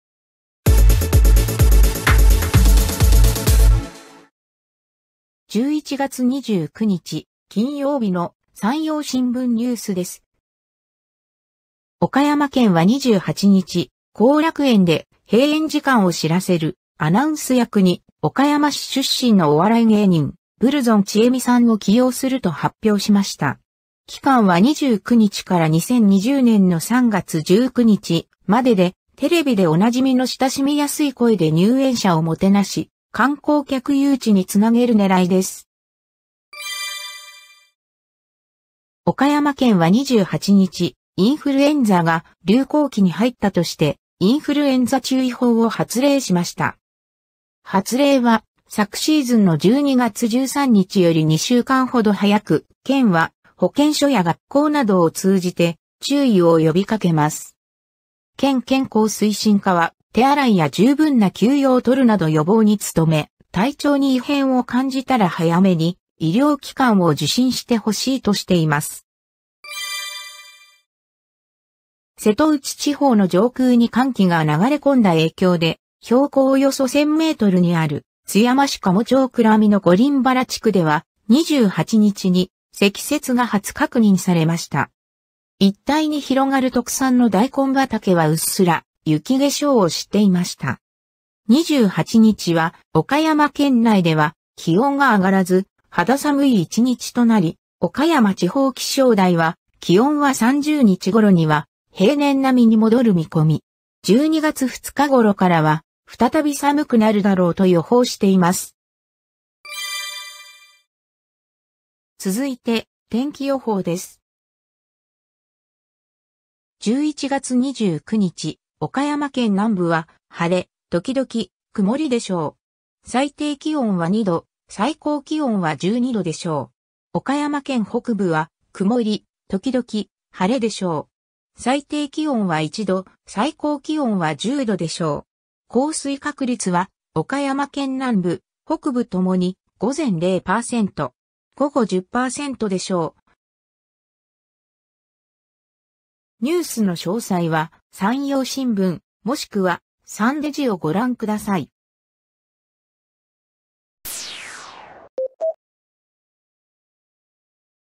【１１月２９日】後楽園アナにブルゾンさん ＡＩアナＮＥＷＳ
後楽園アナにブルゾンさん
インフルエンザ注意報発令
一面うっすら雪化粧